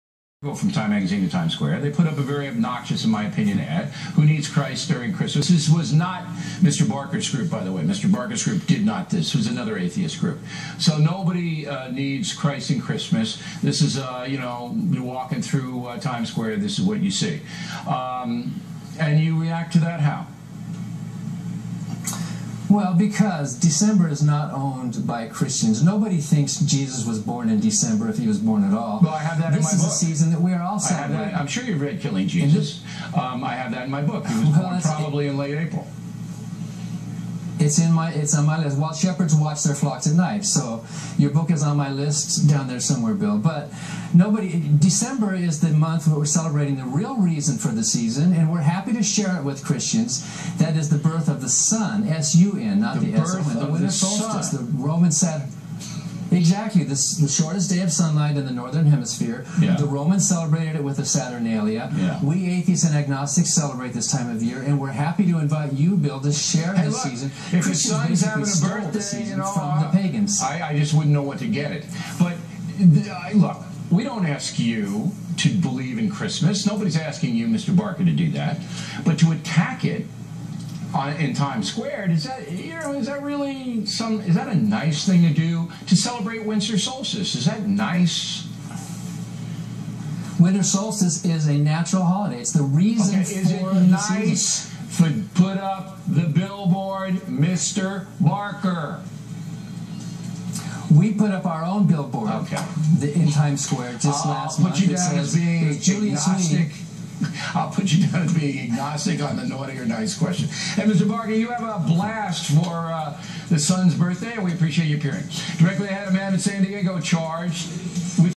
All-out atheist attack on Christ in Times Square
Atheist.org runs anti-Christ ad targeted toward Christmas, to take the Christ out of Christmas. Personally I think this is a HateCrime Christmas is about Jesus's birth. Lawyers start fighting back